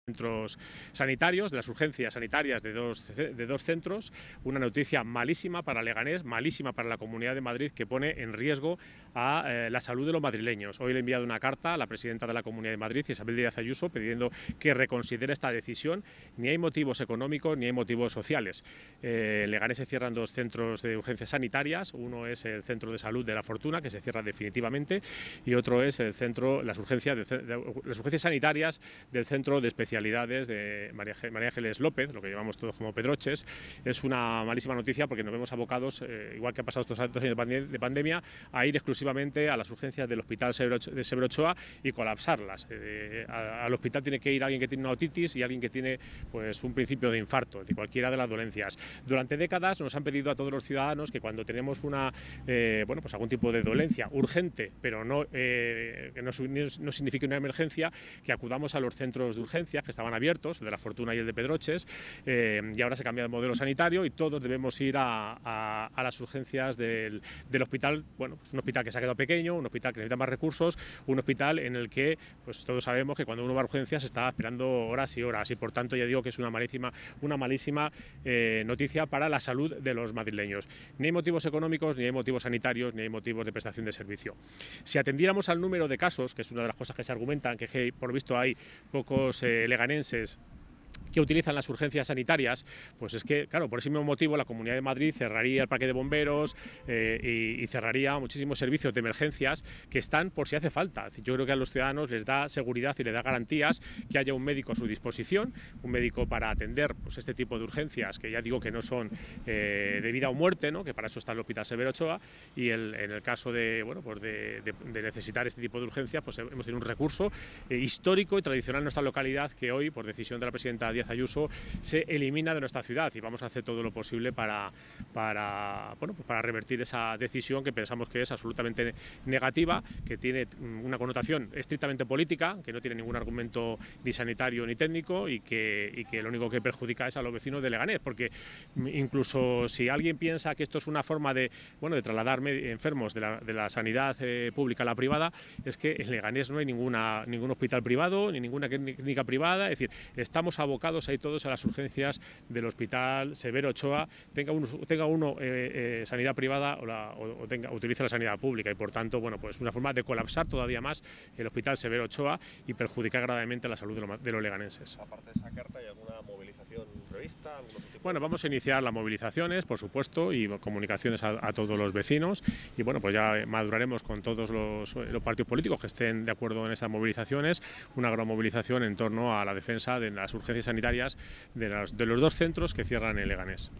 LEGANES reclama reconsiderar el CIERRE de urgencias SUAP
El alcalde de Leganés, Santiago Llorente, ha enviado una carta a la presidenta en la que acusa a Ayuso de “hacer oídos sordos” a las necesidades de la ciudadanía de Leganés, que durante estos últimos meses viene reclamando la apertura de un servicio que se ha demostrado “tan necesario, tan eficaz y tan eficiente para todos”.